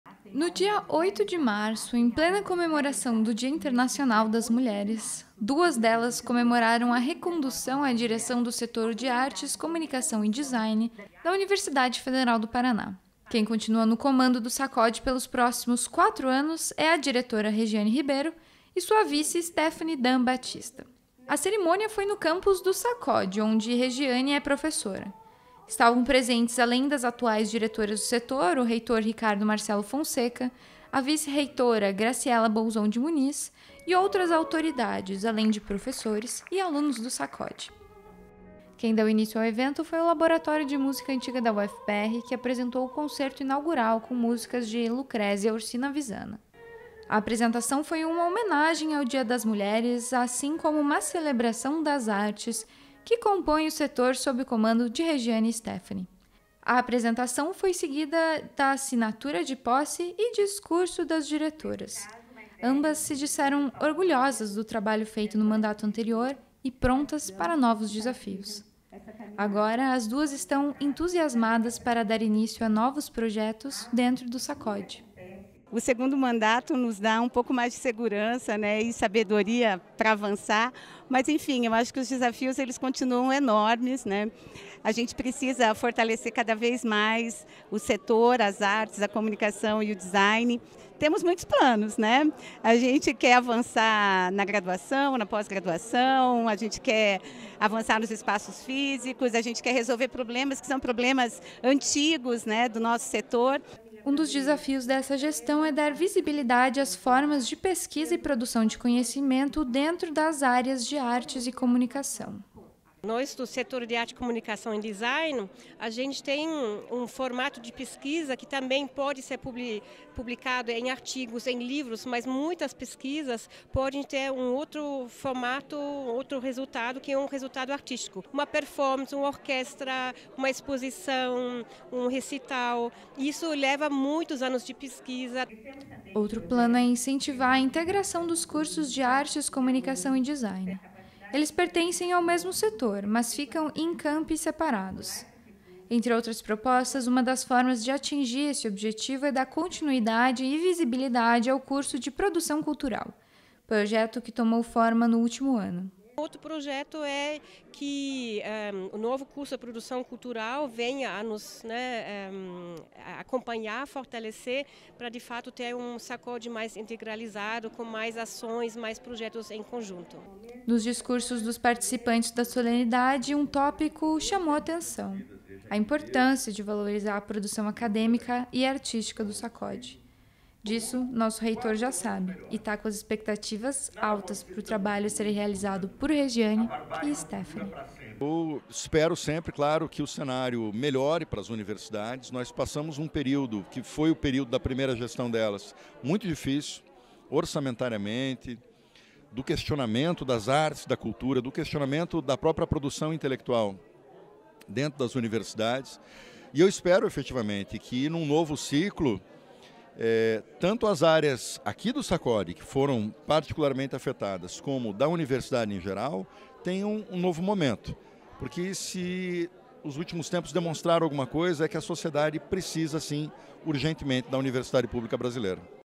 Informa | Direção do Sacod UFPR é reconduzida ao cargo
Na última terça-feira (8), o Setor de Artes, Comunicação e Design (Sacod) da UFPR celebrou a recondução ao cargo da Diretora Regiane Ribeiro e da Vice-Diretora Stephanie Batista. Dentre os desafios da gestão, estão integrar os cursos pertencentes ao Setor, hoje localizados em campi separados, e dar visibilidade às pesquisas desenvolvidas.